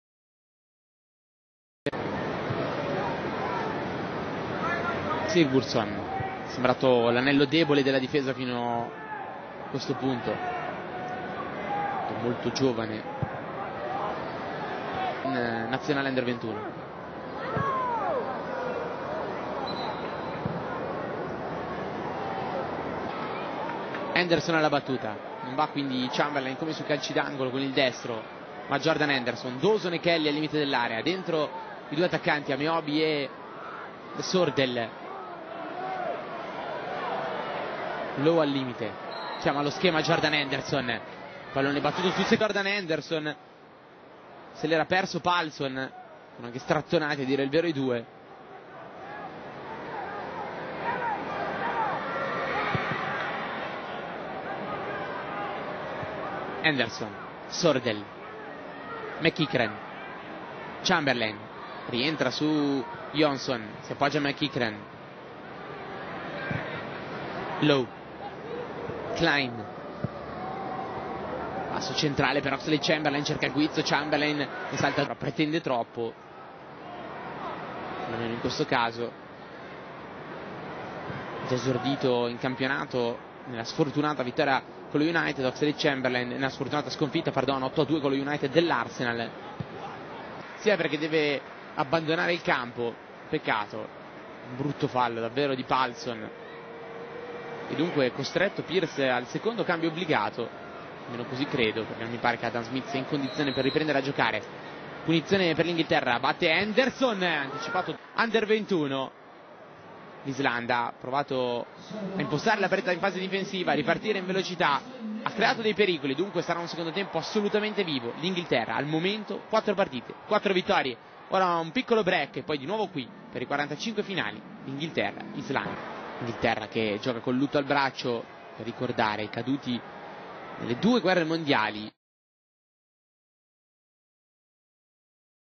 Gudmundur Thorarinsson moments against England
Who: Gudmundur Thorarinsson
Born: 1992
Position: Midfield (left, center)
What: England vs. Iceland - U21 friendly